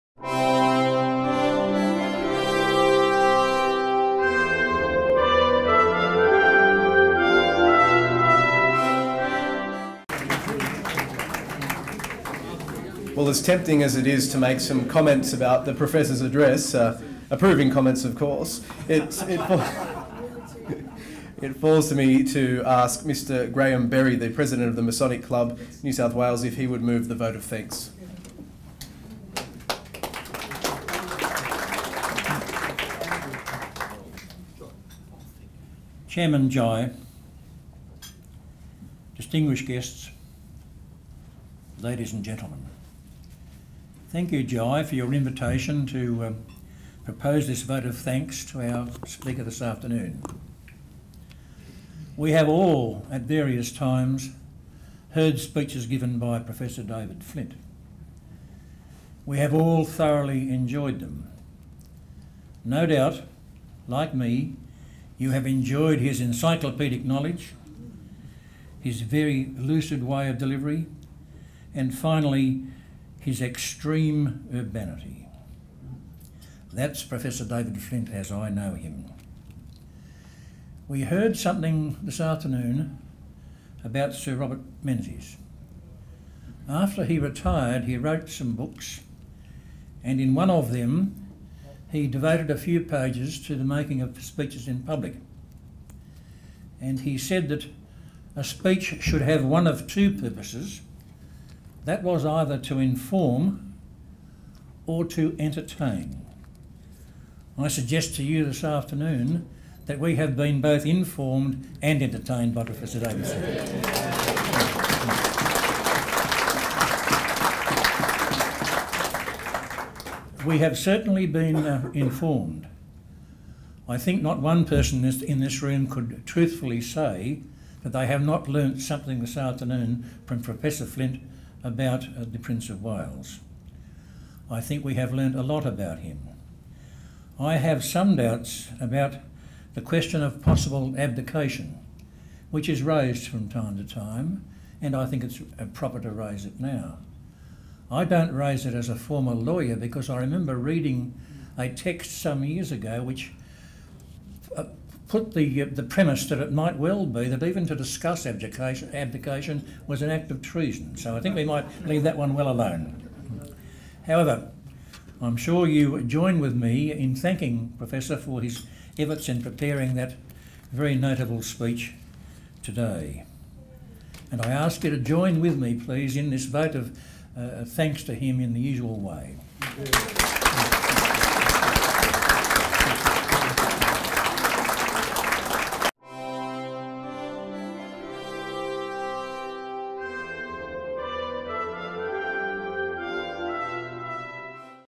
Vote of Thanks to Professor David Flint
Mr. Graham Berry, President of The Masonic Club of New South Wales, moves the Vote of Thanks to Professor David Flint on his address, " In Praise of The Prince of Wales" on The Prince's 64th birthday.